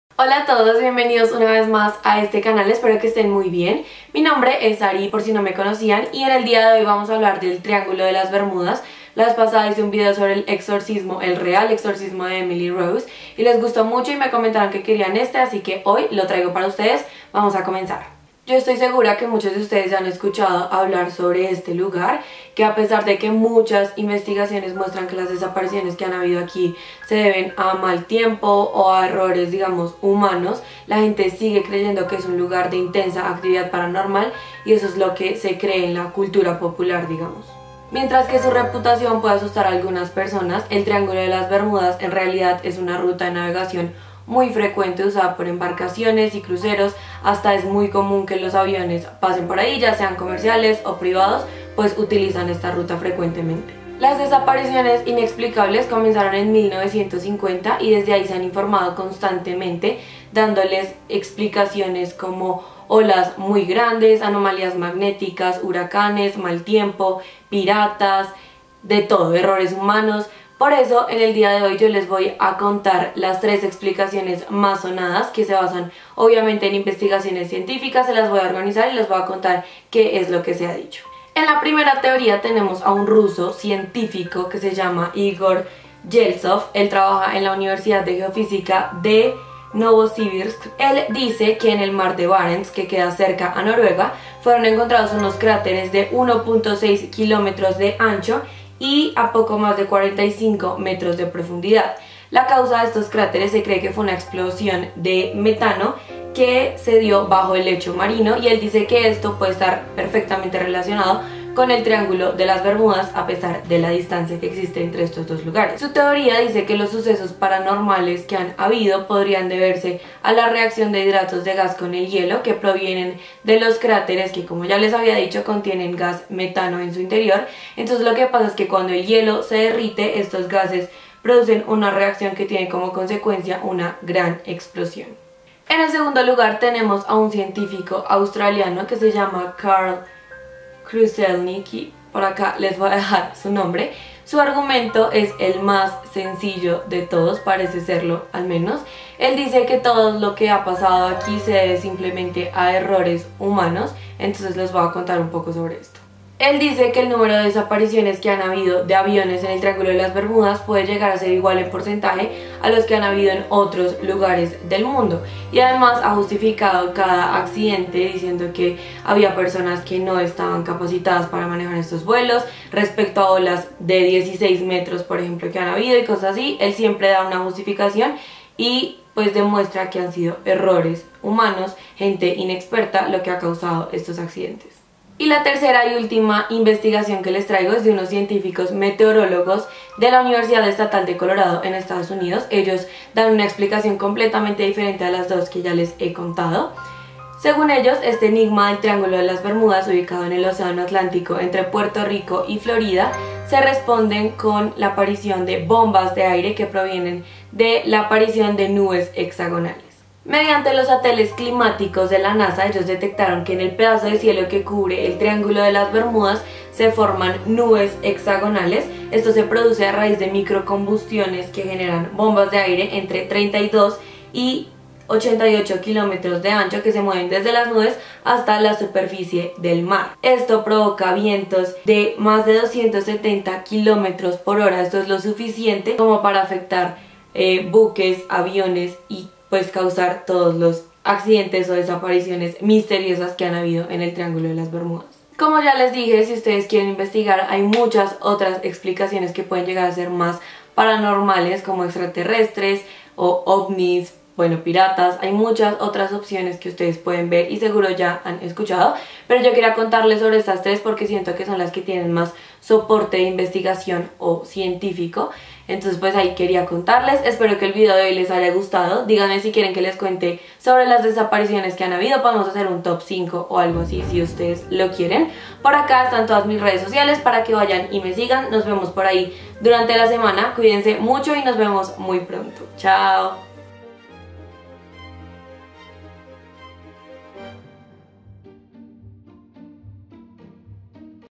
ENCONTRÉ INFORMACIÓN CONFIDENCIAL DEL TRIÁNGULO DE LAS BERMUDAS
INFORMACIÓN CONFIDENCIAL DEL TRIÁNGULO DE LAS BERMUDAS

Aquí les cuento varias teorías que han intentado resolver el gran misterio del triángulo de las bermudas y las desapariciones de aviones, barcos y tripulaciones. Escríban en los comentarios si quieren que haga un video contándoles el TOP5 de desapariciones misteriosas.